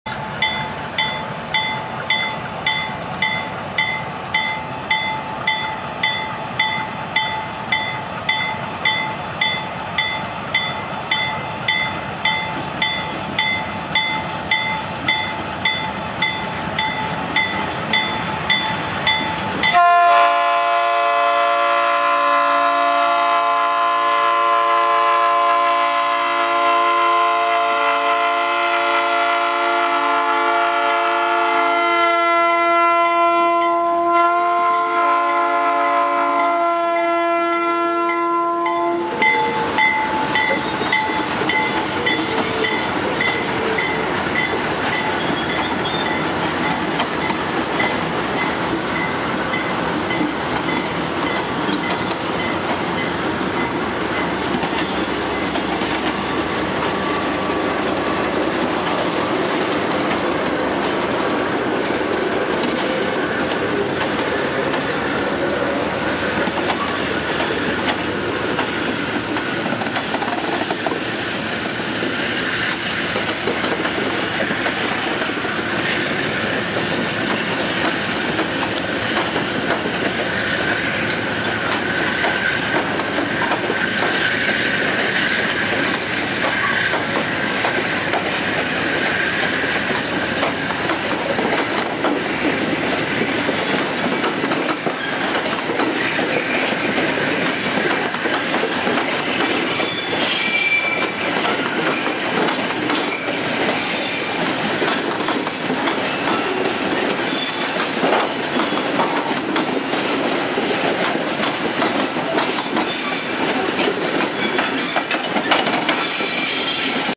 BB36 VLi EFVM CSX CONRAIL
Trem de carga com locomotivas BB36 , de passagem por Embú Guaçú SP, locomotivas na pintura EFVM , CSX e Conrail.